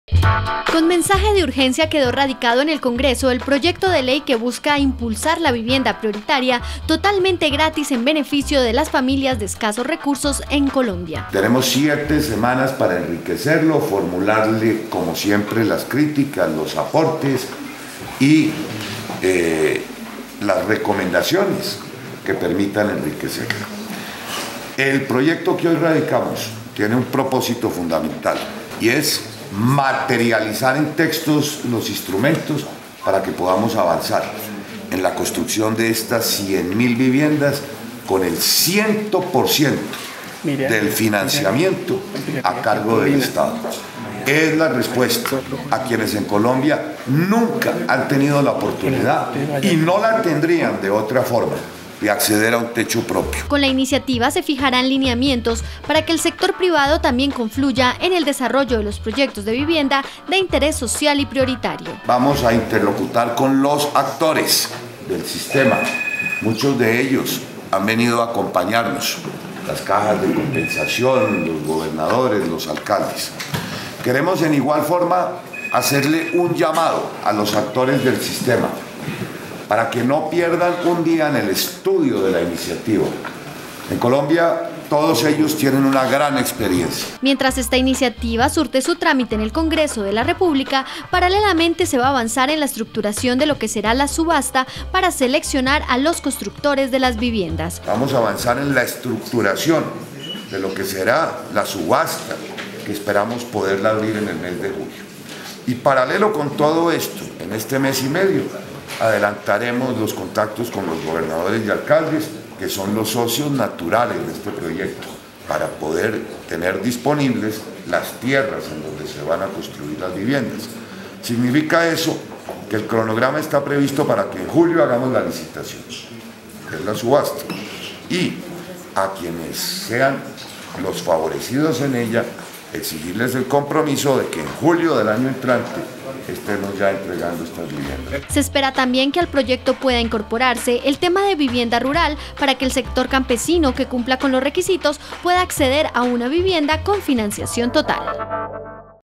Se radica proyecto de ley VIS
El designado Ministro de Vivienda, Ciudad y Territorio, Germán Vargas Lleras, radicó ante la Secretaria General de la Cámara de Representantes, el proyecto de ley que busca facilitar y promover el desarrollo urbano y el acceso a la vivienda de interés social prioritario totalmente gratis para los más pobres de los pobres.